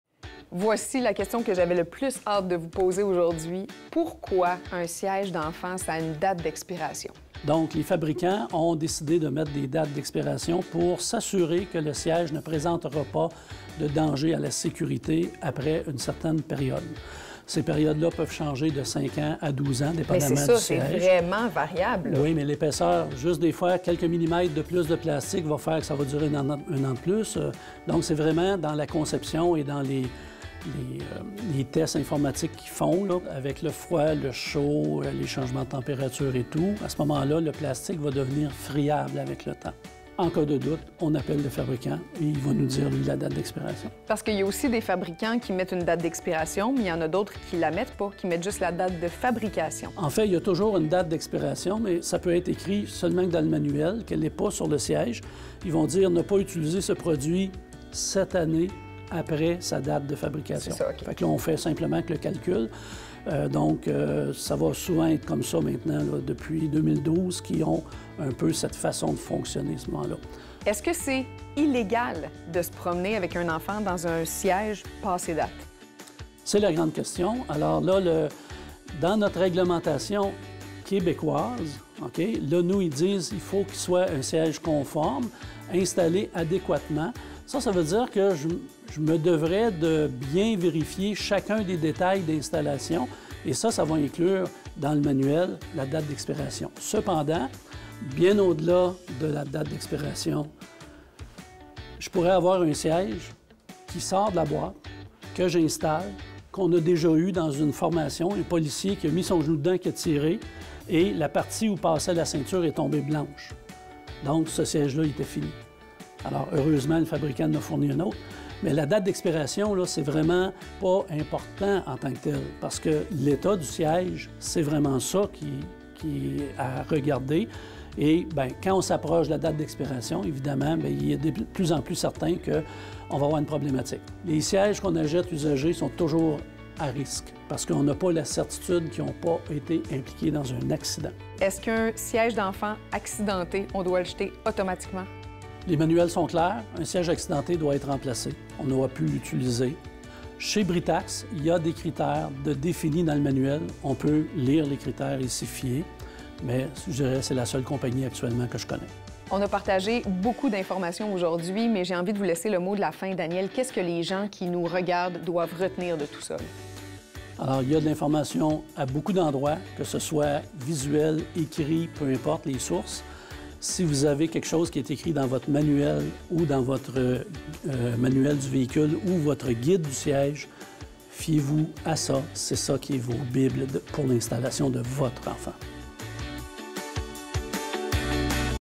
Siège d'enfant dans la voiture - une loi québécoise
Evelyne Audet rencontre Daniel Fortier, formateur et spécialiste de CAA Québec pour nous expliquer pourquoi il y a des dates de péremption sur les sièges d'enfants.